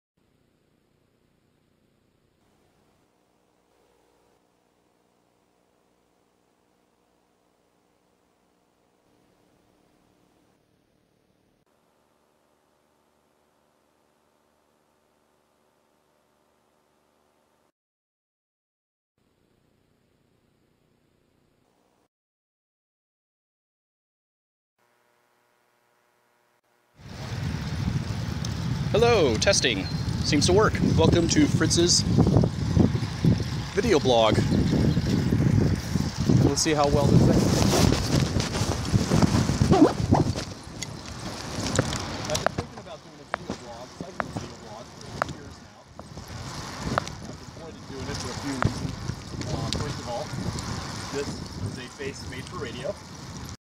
My first ever vlog attempt -- only 53 seconds!
Attempting to video blog while cycling is harder than it may appear.